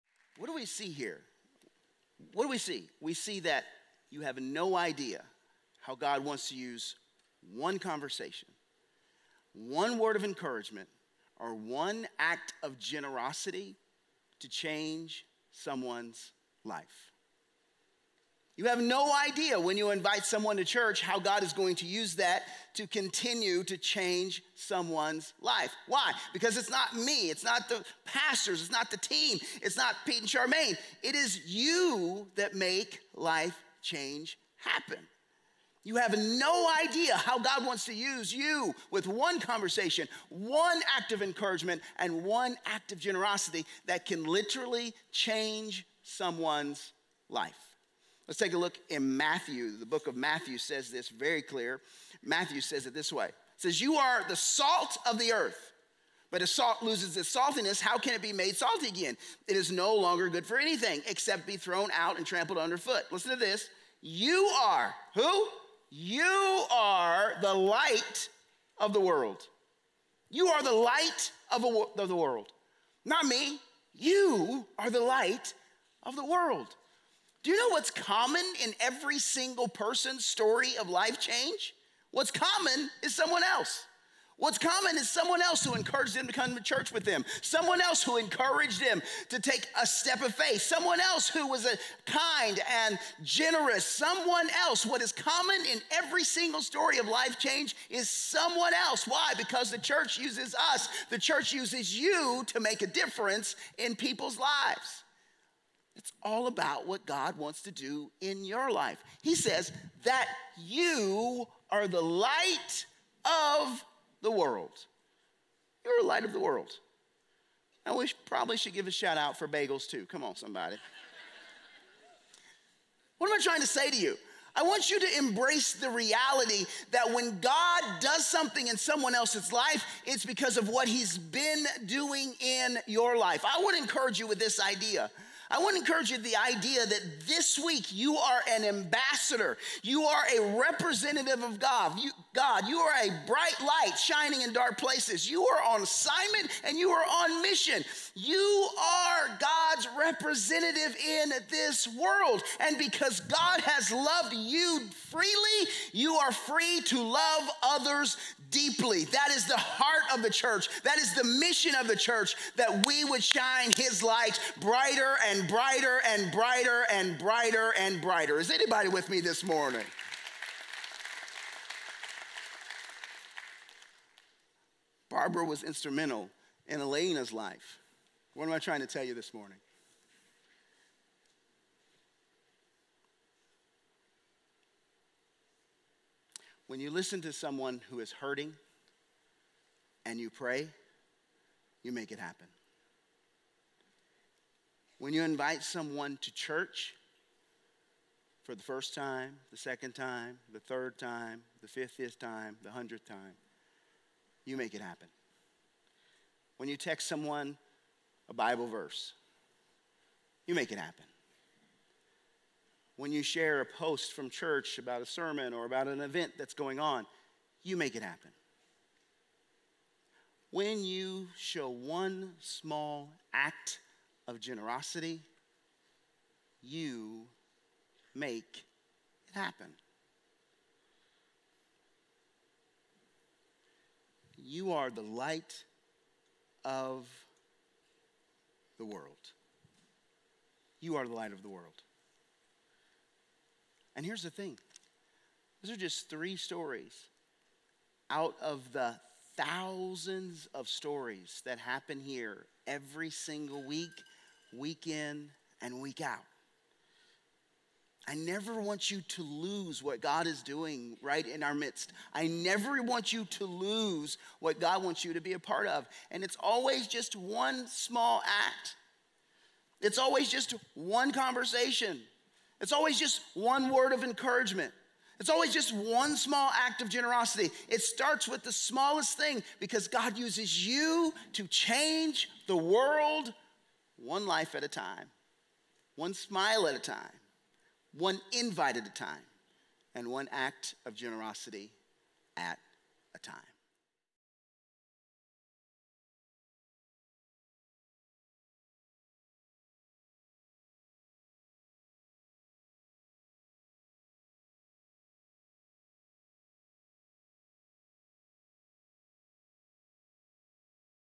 You Are The Light
What does God want to do in your life? God has loved you freely, so you are free to love others like this.